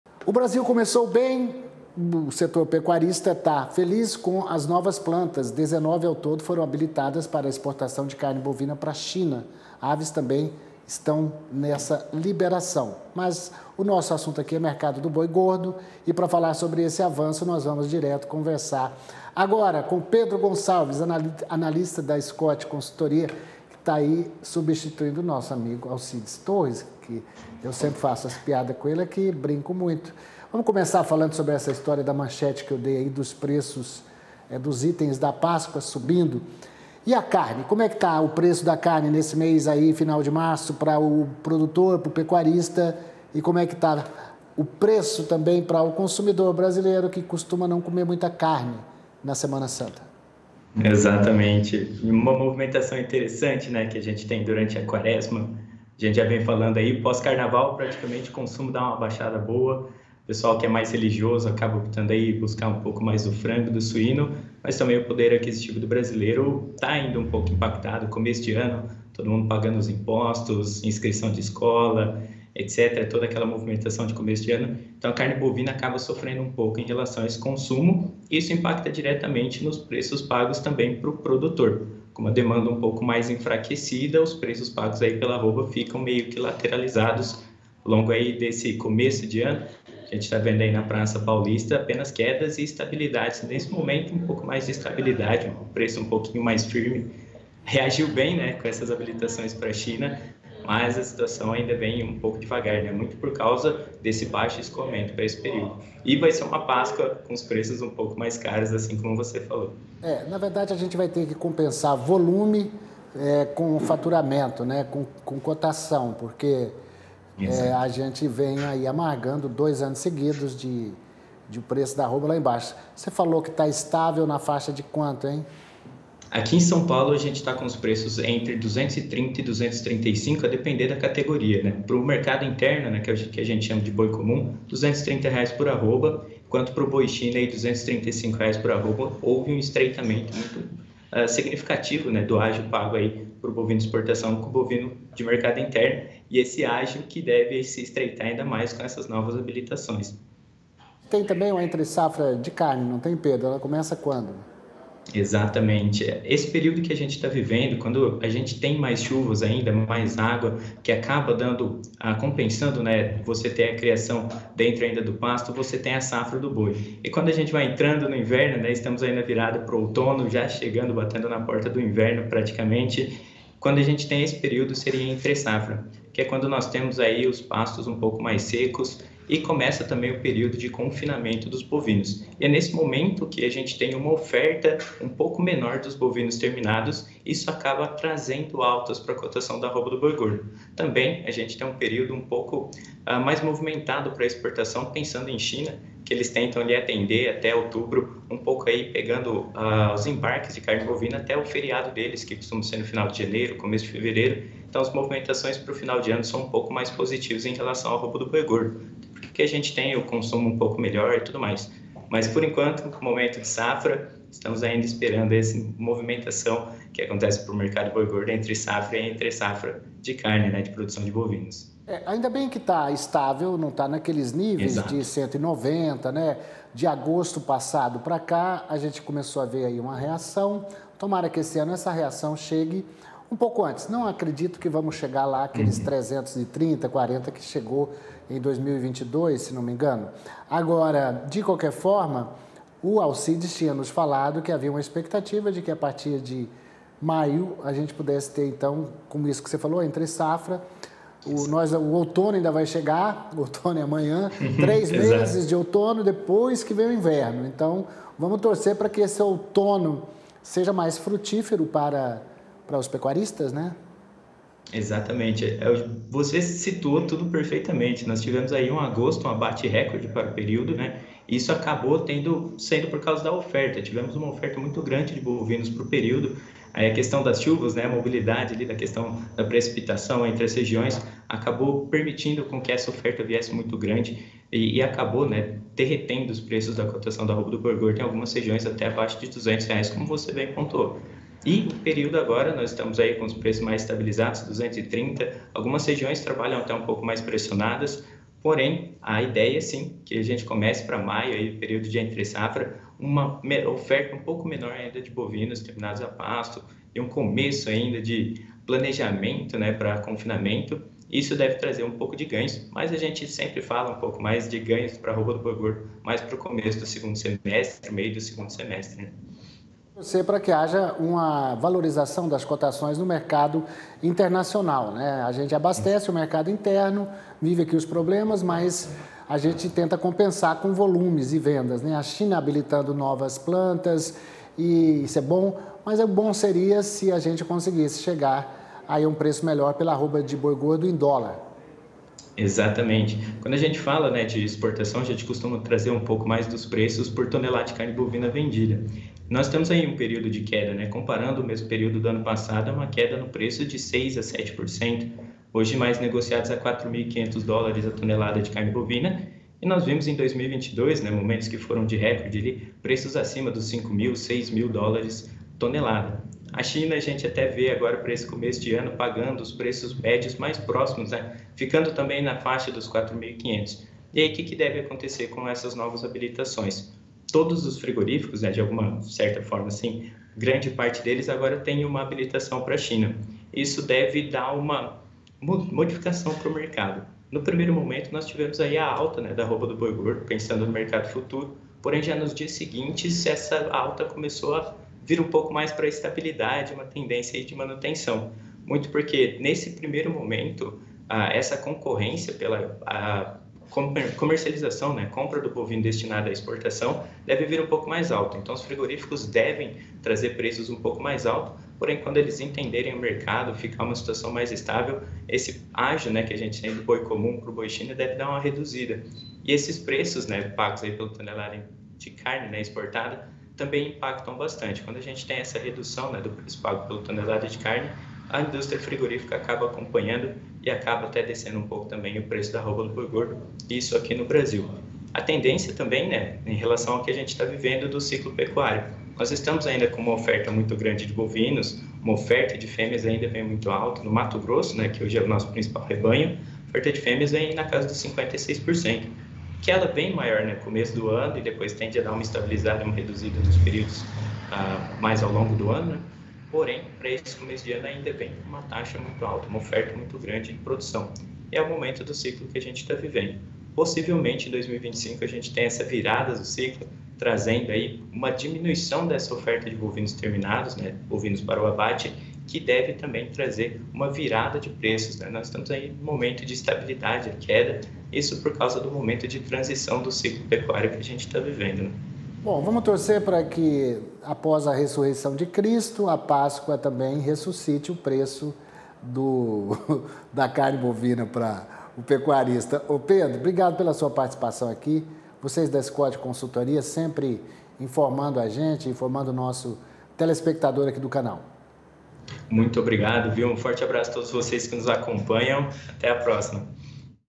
Scot Consultoria analisa habilitação de novas plantas frigoríficas
O Brasil começou bem, o setor pecuarista está feliz com as novas plantas. 19, ao todo, foram habilitadas para exportação de carne bovina para a China. Para falar sobre esse avanço, conversamos com Pedro Gonçalves, analista da Scot Consultoria.